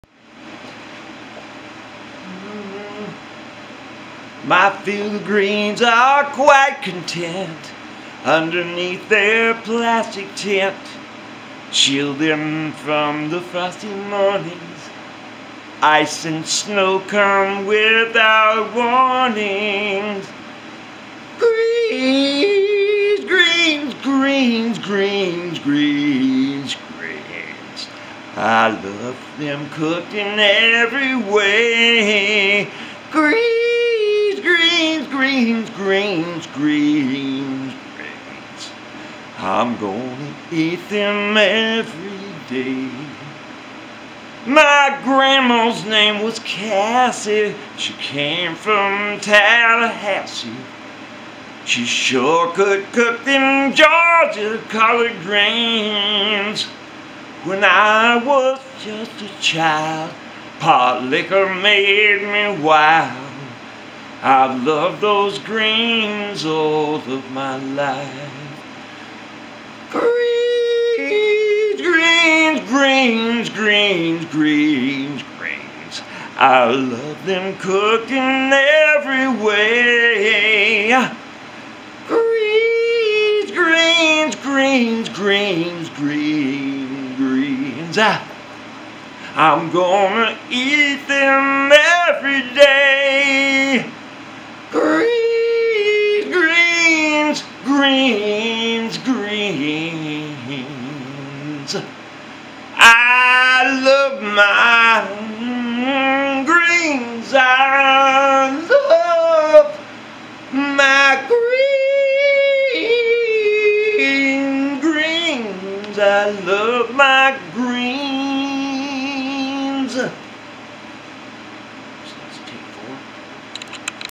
Vocal 2 Greens Take IV For Singing Masterclass 12 5 21
A song evolves I wrote just before singing class yesterday.